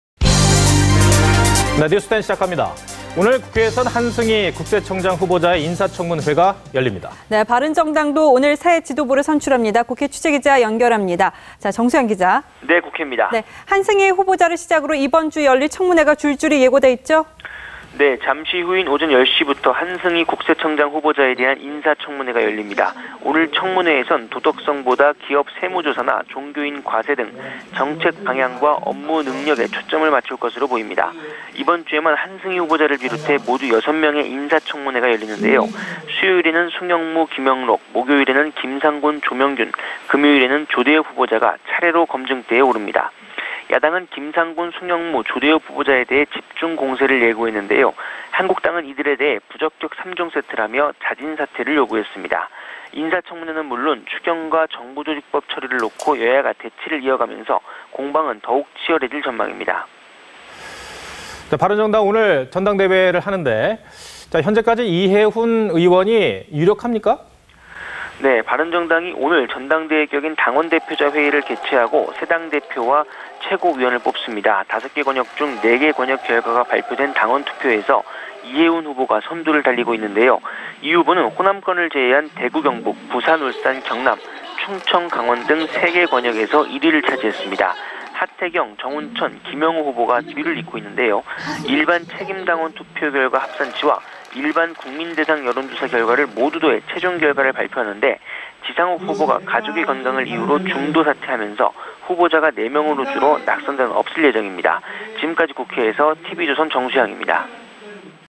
한승희 인사청문회 개최…바른정당 지도부 선출
오늘 국회에선 한승희 국세청장 후보자의 인사 청문회가 열립니다. 바른정당도 오늘 새 지도부를 선출합니다.